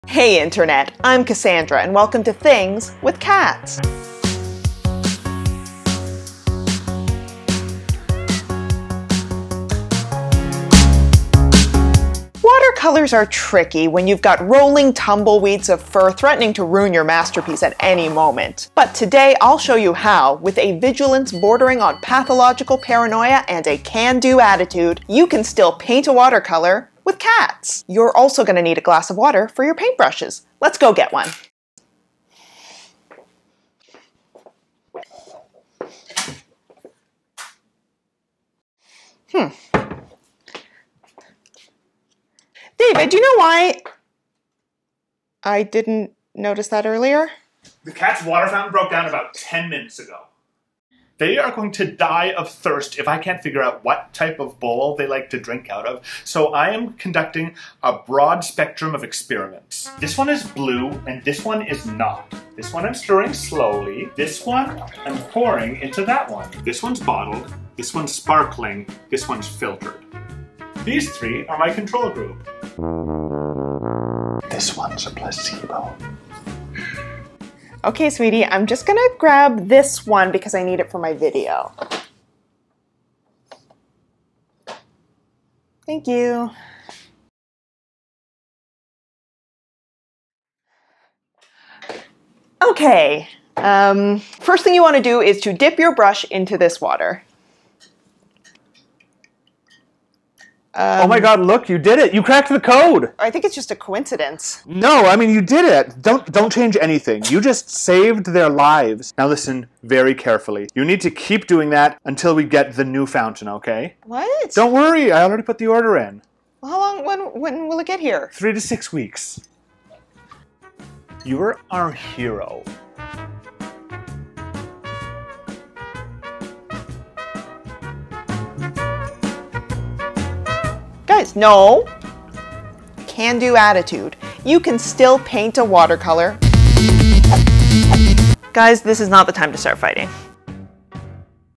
WATERCOLOURS WITH CATS turns into WATER FOR CATS when their fountain breaks! | THINGS WITH CATS
Cassandra tries to take on watercolours this week, but gets derailed by David's... whatever David is doing.

Have we spoiled our cats by buying them multiple drinking fountains? Yes.
Have each of these fountains either broken or turned into cesspools of wet hair? Also yes.
Would our rather drink out of a tupperware container? Probably!
Is this video an unauthorized remake of the 2011 film Water for Elephants starring Robert Pattinson, Reese Witherspoon, and Christoph Waltz? It's impossible to tell!

Written by Josef Beeby and Grace Gordon
Starring Grace Smith, Leete Stetson, Kitty and Moira
Edited by  Grace Smith